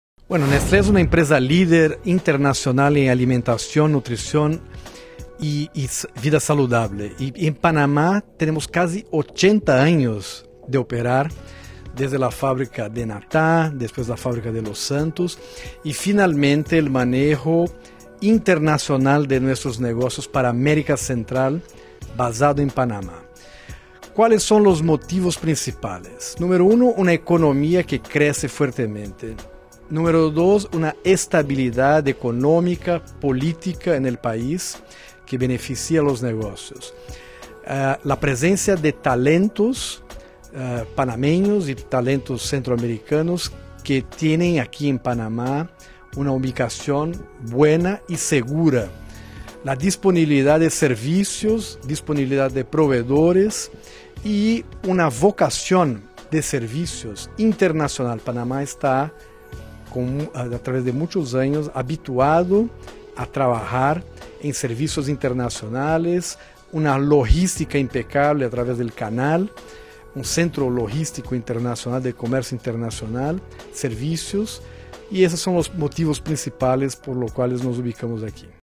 Leo Leiman, Presidente y CEO de Néstle Centroamérica
¿Por qué Panamá es LaGranConexión? Leo Leiman, Presidente y CEO de Néstle Centroamérica nos dice el por qué de elegir Panamá.